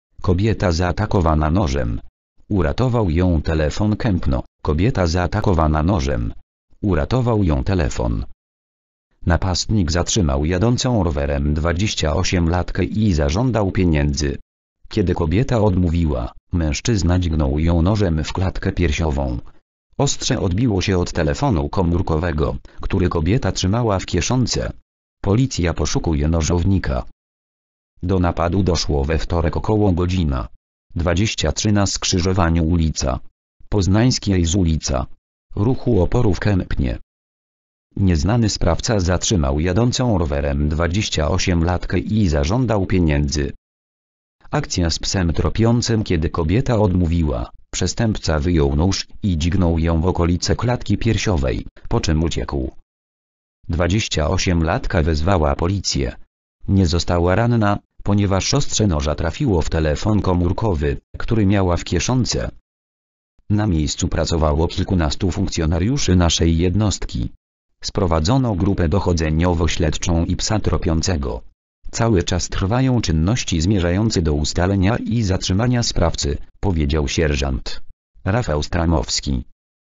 Kobieta zaatakowana nożem. Uratował ją telefon.
Kępno: Kobieta zaatakowana nożem. Uratował ją telefon.

    (fot. Pixabay/wyncel - zdjęcie ilustracyjne)